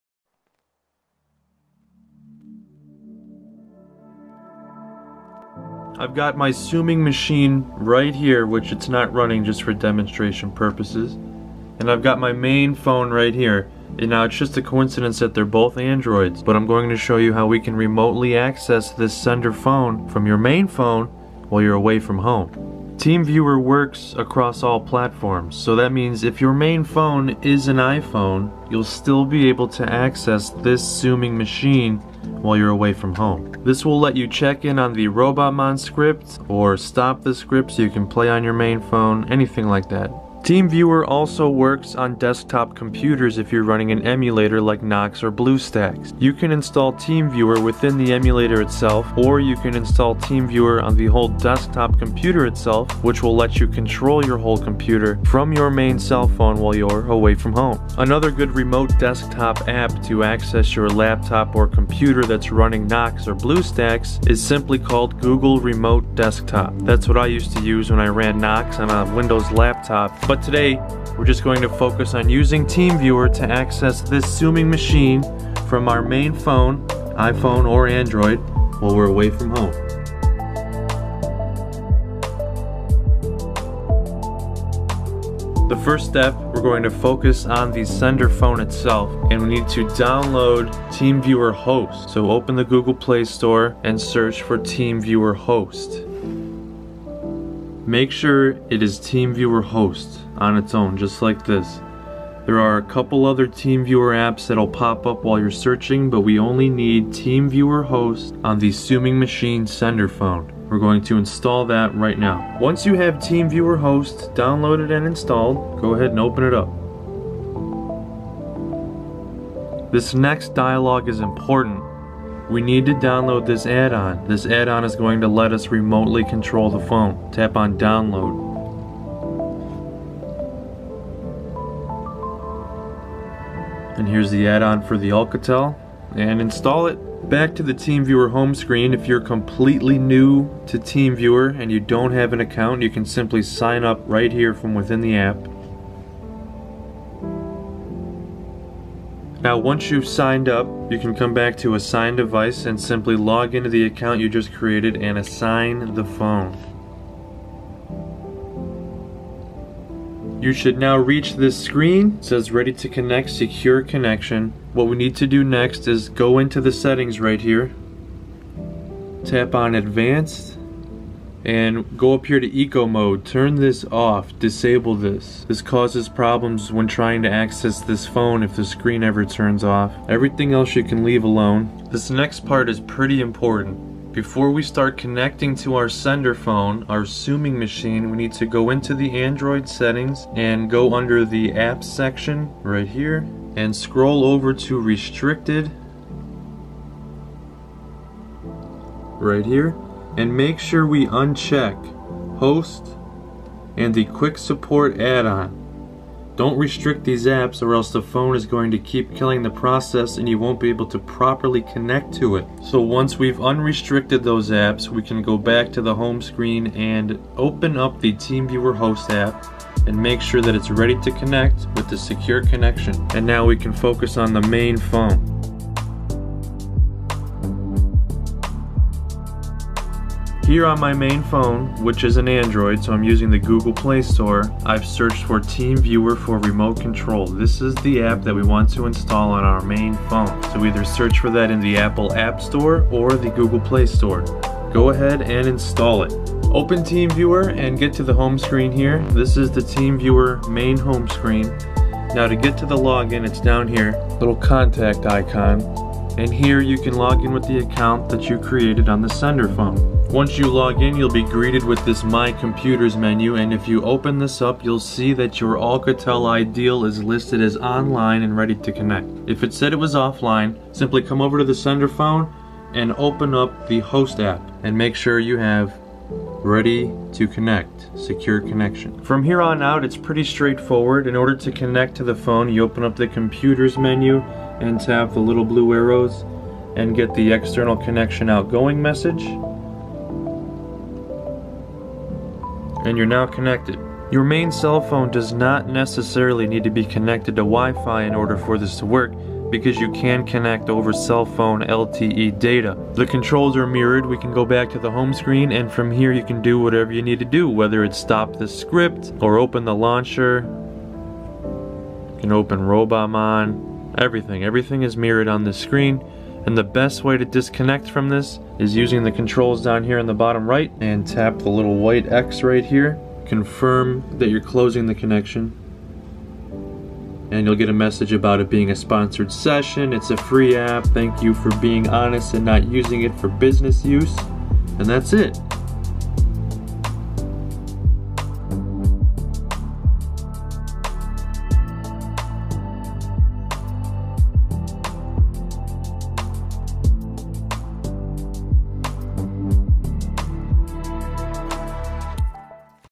Don’t Lose Control—Access Your Android Remotely Anytime, Anywhere!
In this quick tutorial, I’ll show you how to use TeamViewer to securely access your Android device, iPhone, or computer over the internet. Whether you're traveling, troubleshooting for friends, or just managing devices from afar, remote access is a game-changer. I used this while running "Tsum Tsum Autosending" using "Robotmon", but this phone model is now discontinued.

- How to install TeamViewer on Android, iOS, and desktop

- How to set up remote access from anywhere (no root required)

Perfect for remote work, tech support, or staying connected on the go.